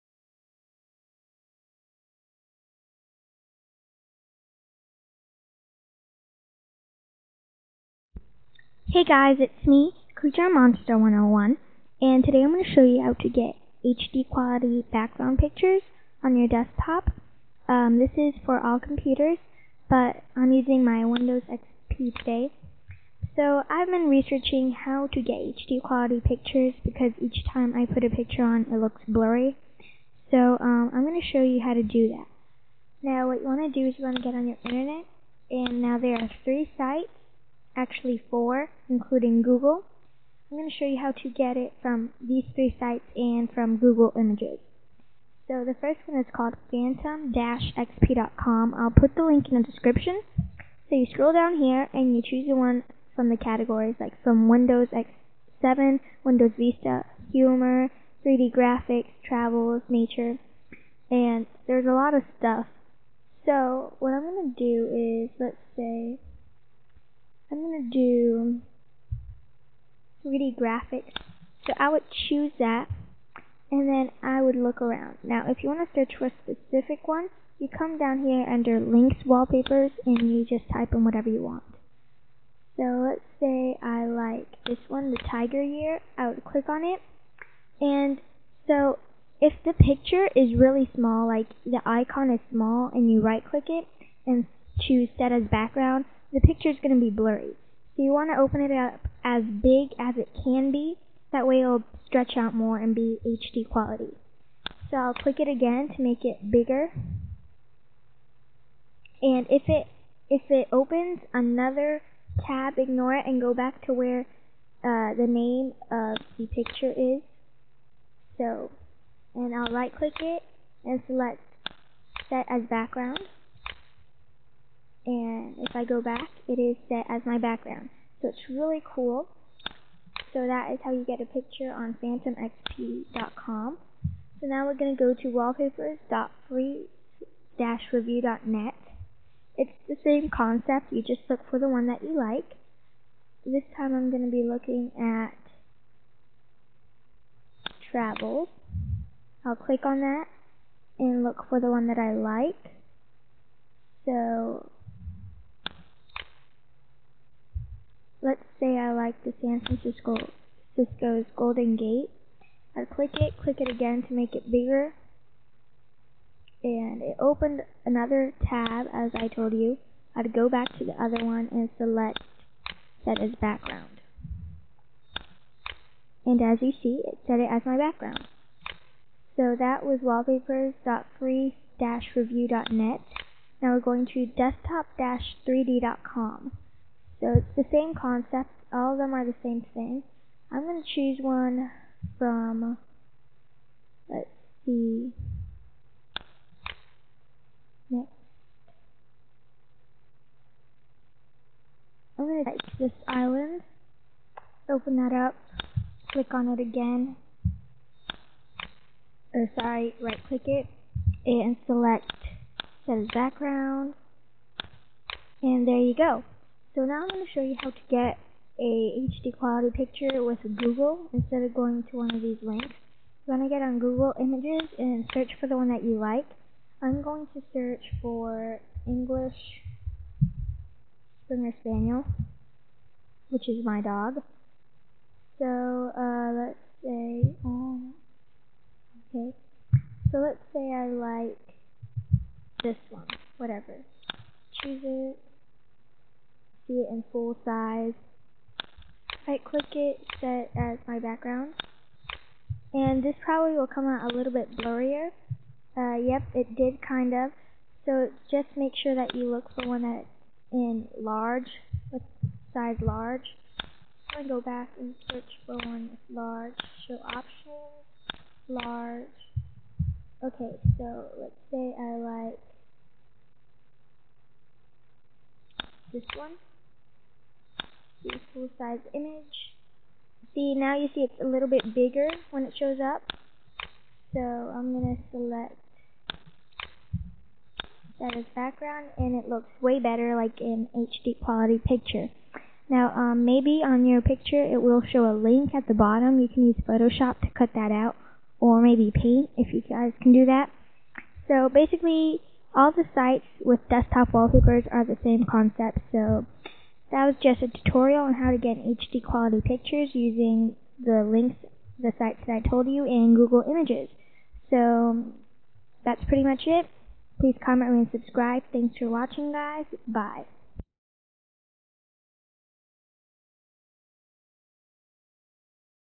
How to get a HD Quality Wallpaper
How to get a HD Quality Wallpaper on your computer!!!

Internet Explorer Users: Right-click your mouse on the wallpaper and select 'Set As Background'.
FireFox Users: Right-click your mouse on the wallpaper and select 'Set As Desktop Background'. Then click on the 'Set Desktop Background' button.
Mac Users: Drag and drop the wallpaper onto your desktop. Do this by clicking your mouse on the wallpaper, hold down the button while you drag your mouse onto your desktop, then release the mouse button. Your desktop will have to be visible on your screen so you can drag your mouse onto it.